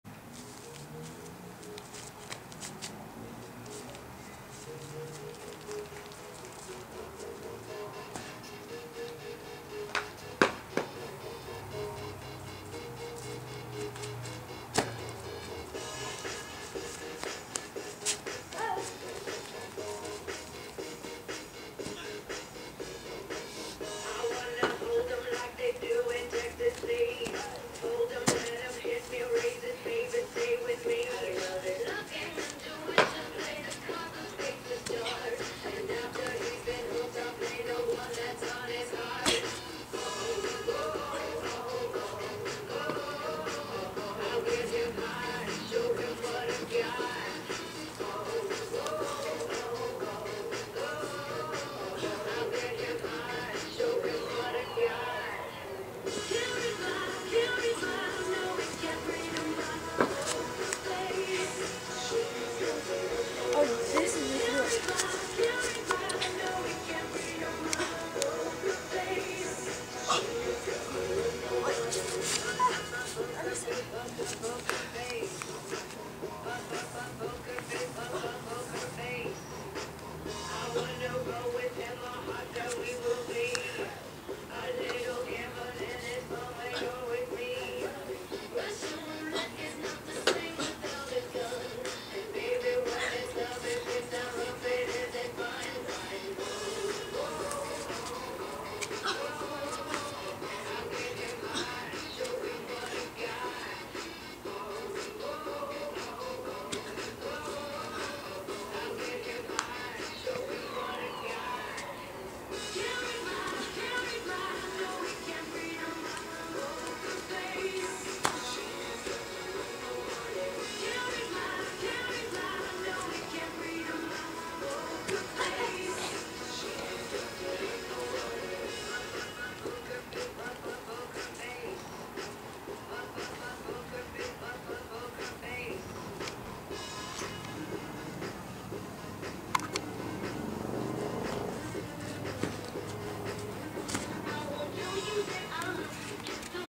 Giselle's Backyard Gymnastics
Giselle fooling around in the backyard with some gymnastic stunts.